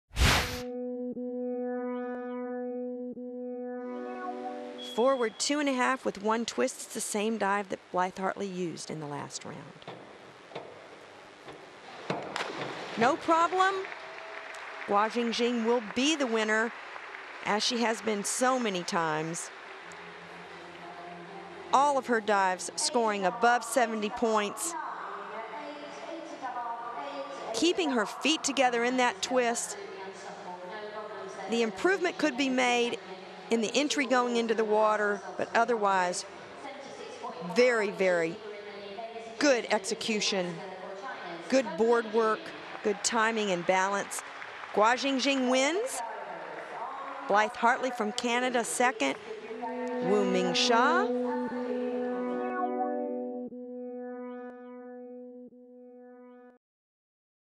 More Gold for Jing Jing Guo
Chinese diver Jing Jing Guo scored her 4th gold medal of the season, with a combine score of 380.45 on the springboard. At the Fina Diving World Series 2008 in Sheffield, Great Britain. (WCSN, WCSN.com)